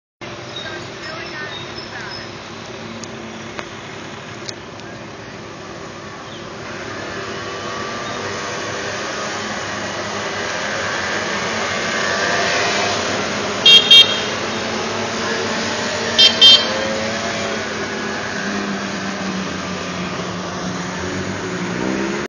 Eight girls on a scooter rally in Denver Colorado Summer 2013 ScooTours Denver Scooter Rental
Three of these eight girls on the mini scooter rally rented Kymco People 50 light blue scooters from ScooTours Denver.